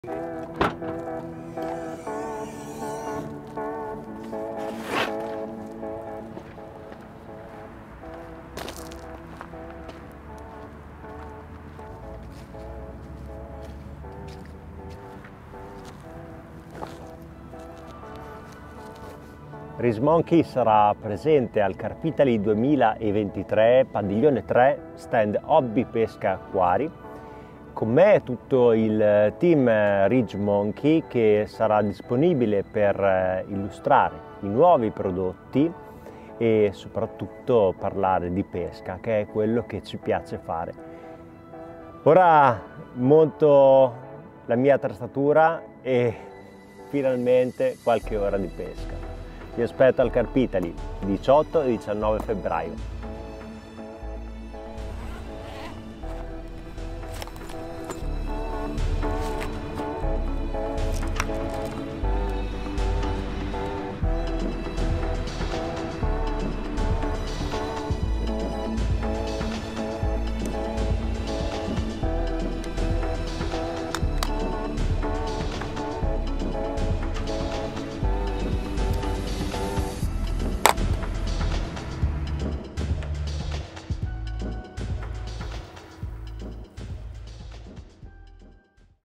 RidgeMonkey Italia - Carpitaly 2023: vi aspettiamo!
RidgeMonkey Italia vi aspetta al Carpitaly 2023 per illustrare gli esclusivi prodotti e per poter parlare con i nostri collaboratori.
La fiera iconica italiana per la pesca alla carpa torna con grandi obiettivi! Noi saremo pronti a parlare di pesca in un contesto che ha fatto la storia del mondo del carp fishing in Italia.
Vi aspettiamo, Pad 3!